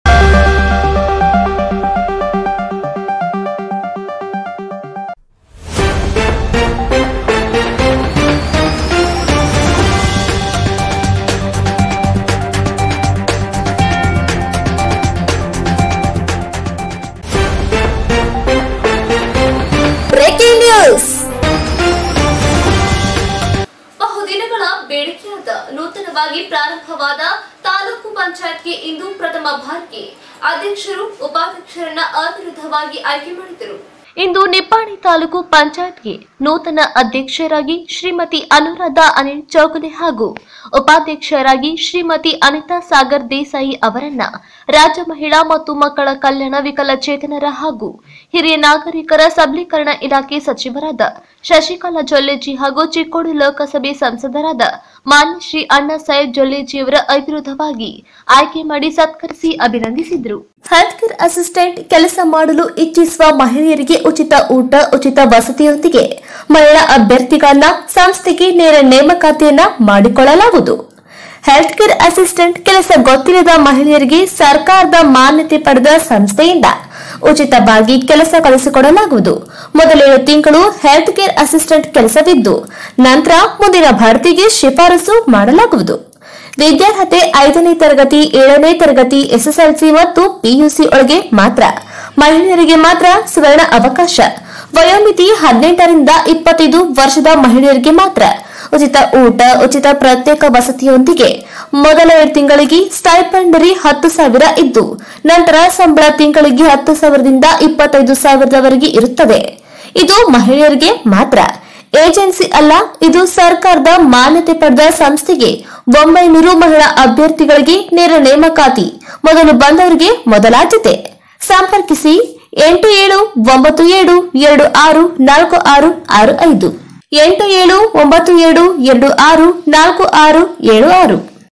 ನೂತನವಾಗಿ ಪ್ರಾರಂಭವಾದ ತಾಲೂಕು ಪಂಚಾಯತ್ ಗೆ ಪ್ರಥಮ ಬಾರಿಗೆ ಅಧ್ಯಕ್ಷರು, ಉಪಾಧ್ಯಕ್ಷರನ್ನು ಆಯ್ಕೆ
ಅವಿನ್ ಟಿವಿ ಸುದ್ದಿ ವಾಹಿನಿಯಲ್ಲಿ ಕಾರ್ಯನಿರ್ವಹಿಸಲು ಜಿಲ್ಲಾ/ ತಾಲುಕಾ ಅನುಭವಿ ವರದಿಗಾರರ ಬೇಕಾಗಿದ್ದಾರೆ

ಸಂಪರ್ಕಿಸಿ

ನಿಪ್ಪಾಣಿ
ಬಹುದಿನಗಳ ಕನಸು ಸಾಕಾರ.

ಬಹುದಿನಗಳ ಬೇಡಿಕೆಯಾಗಿದ್ದ ನಿಪ್ಪಾಣಿ ತಾಲೂಕು ರಚನೆಯ ಕನಸು ಈಗಾಗಲೇ ನನಸಾಗಿದೆ. ನೂತನವಾಗಿ ಪ್ರಾರಂಭವಾದ ತಾಲೂಕು ಪಂಚಾಯತ್ ಗೆ ಇಂದು ಪ್ರಥಮ ಬಾರಿಗೆ ಅಧ್ಯಕ್ಷರು, ಉಪಾಧ್ಯಕ್ಷರನ್ನು ಅವಿರೋಧವಾಗಿ ಆಯ್ಕೆ ಮಾಡಿದರು.

ಇಂದು ನಿಪ್ಪಾಣಿ ತಾಲೂಕು ಪಂಚಾಯತ್ ಗೆ  ನೂತನ ಅಧ್ಯಕ್ಷರಾಗಿ ಶ್ರೀಮತಿ ಅನುರಾಧಾ ಅನೀಲ ಚೌಗಲೆ ಹಾಗೂ ಉಪಾಧ್ಯಕ್ಷರಾಗಿ ಶ್ರೀಮತಿ ಅನೀತಾ ಸಾಗರ ದೇಸಾಯಿ ಅವರನ್ನು ರಾಜ್ಯ ಮಹಿಳಾ ಮತ್ತು ಮಕ್ಕಳ ಕಲ್ಯಾಣ, ವಿಕಲಚೇತನರ ಹಾಗೂ ಹಿರಿಯ ನಾಗರಿಕರ ಸಬಲೀಕರಣ ಇಲಾಖೆ ಸಚಿವರಾದ ಸೌ. ಶಶಿಕಲಾ ಜೊಲ್ಲೆ ಜಿ, ಹಾಗೂ ಚಿಕ್ಕೋಡಿ ಲೋಕಸಭೆ ಸಂಸದರಾದ ಮಾನ್ಯ ಶ್ರೀ ಅಣ್ಣಾಸಾಹೇಬ ಜೊಲ್ಲೆ ಜಿ ಯವರು ಅವಿರೋಧವಾಗಿ ಆಯ್ಕೆಮಾಡಿ, ಸತ್ಕರಿಸಿ, ಅಭಿನಂದಿಸಿದರು.

ಈ ಸಂದರ್ಭದಲ್ಲಿ ಉಪವಿಭಾಗಧಿಕಾರಿ ಶ್ರೀ ಯುಕೇಶಕುಮಾರ ಎಸ್, ತಹಶೀಲ್ದಾರರಾದ ಶ್ರೀ ಪ್ರಕಾಶ ಗಾಯಕವಾಡ,ಹಾಲಸಿದ್ಧನಾಥ ಸಕ್ಕರೆ ಕಾರ್ಖಾನೆಯ ಅಧ್ಯಕ್ಷರಾದ ಶ್ರೀ ಚಂದ್ರಕಾಂತ ಕೋಟಿವಾಲೆ, ಉಪಾಧ್ಯಕ್ಷರಾದ ಶ್ರೀ ಎಂ.ಪಿ ಪಾಟೀಲ,ಜಿಲ್ಲಾ ಪಂಚಾಯತ್ ಸದಸ್ಯರು, ತಾಲೂಕ ಪಂಚಾಯತ ಸದಸ್ಯರು, ನಗರಸಭೆ ಸದಸ್ಯರು, ಜನಪ್ರತಿನಿಧಿಗಳು ಹಾಗೂ ಪಕ್ಷದ ಕಾರ್ಯಕರ್ತರು, ಉಪಸ್ಥಿತರಿದ್ದರು.

निपाणी
खूप दिवसांचे स्वप्न साकार.

बर्‍याच दिवसांपासून मागणी असलेल्या निपाणी तालुका निर्मितीचे स्वप्न आधीच साकार झाले आहे. नव्याने सुरू केलेल्या तालुका पंचायतीत आज पहिल्यांदाच अध्यक्ष व उपाध्यक्ष यांची बिनविरोध निवड केली.

आज निपाणी तालुका पंचायतीचे नविन अध्यक्षपदी सौ. अनुराधा अनिल चौगले, आणि उपाध्यक्षपदी सौ. अनिता सागर देसाई, यांना राज्य महिला व बालविकास विभाग,अपंगत्व आणि ज्येष्ठ नागरिकांचे सशक्तिकरण विभागाच्या मंत्री सौ. शशिकला जोल्ले जी(वहिनी) आणि चिक्कोडी लोकसभेचे खासदार माननीय श्री अण्णासाहेब जोल्ले,जी यांनी बिनविरोध निवड करून, सत्कार करून, अभिनंदन केले.

 यावेळी प्रांताधिकारी श्री युकेशकुमार एस, तहसीलदार श्री प्रकाश गायकवाड, हालसिद्धनाथ साखर कारखान्याचे अध्यक्ष श्री चंद्रकांत कोटिवाले, उपाध्यक्ष श्री. एम पी पाटील,जिल्हा पंचायत सदस्य, तालुका पंचायत सदस्य, नगरसेवक, लोकप्रतिनिधी व पक्षाचे कार्यकर्ते उपस्थित होते.